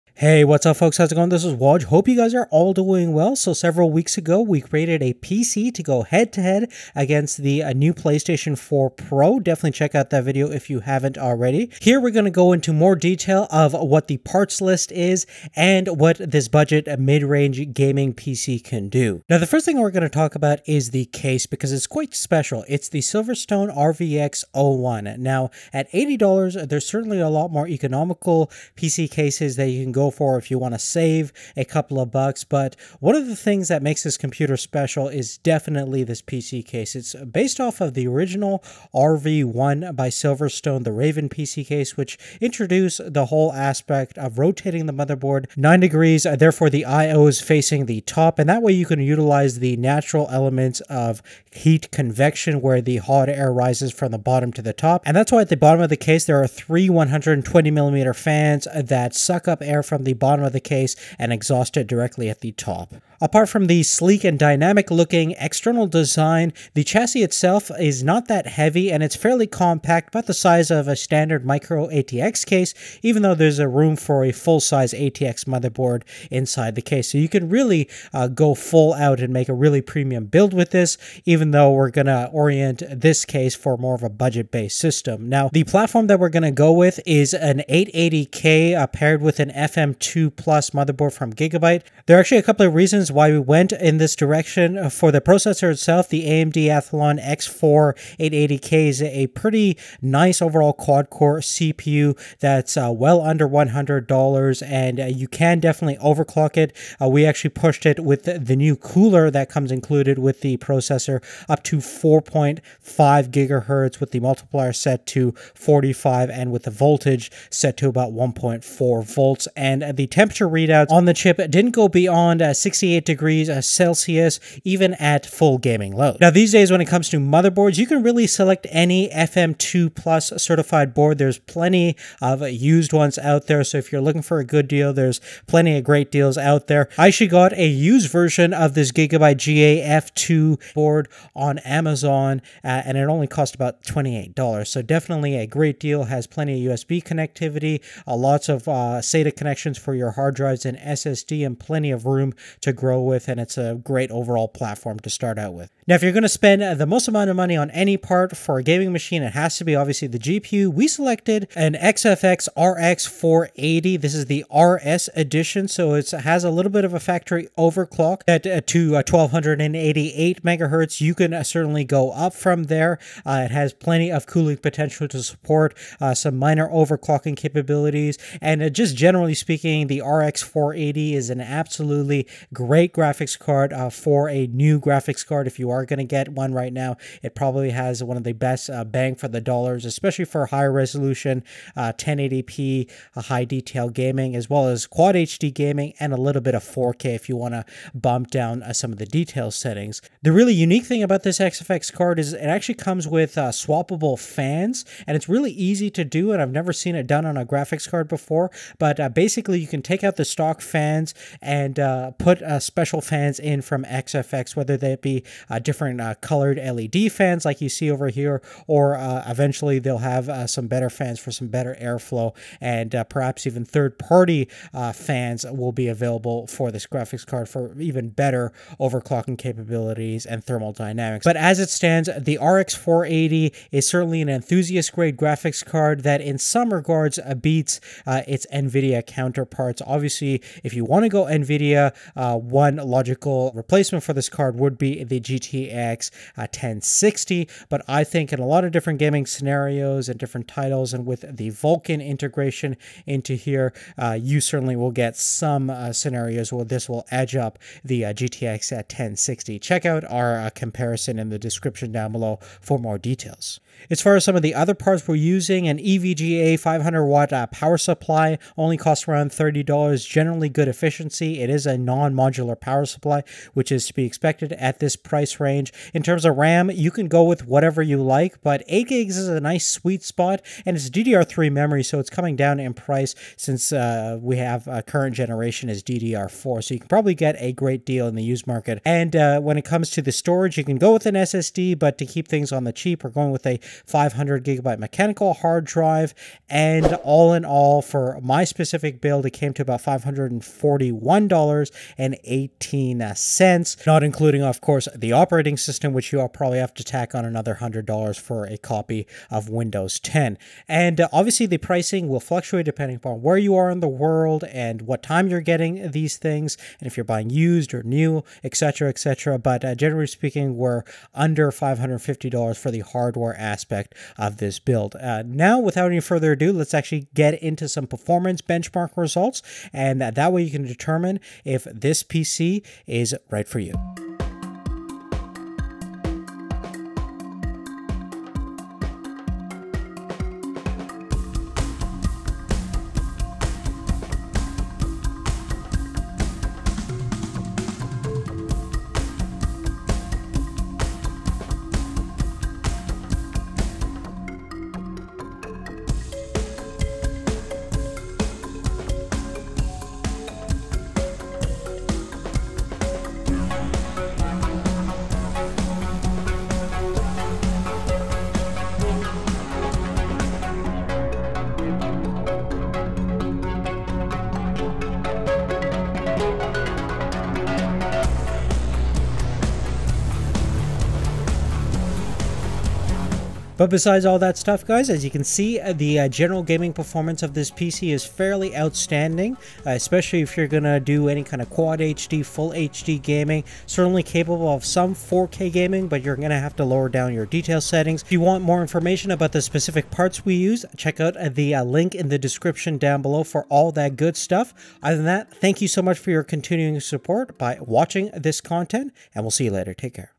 Ultimate Gaming PC Under $550
PC Parts List:
CPU- AMD 880k
Mobo: Gigabyte AMD FM2A68H
GPU- XFX RX480
Ram- 8Gb DDR3
Case- Silverstone RVX01
PSU- 500watt
HDD- 500GB

UK links:
CPU- AMD 880k
GPU- XFX RX480
Ram- 8Gb DDR3
HDD: 500GB
Case: Silverstone RVX01
PSU: 500watt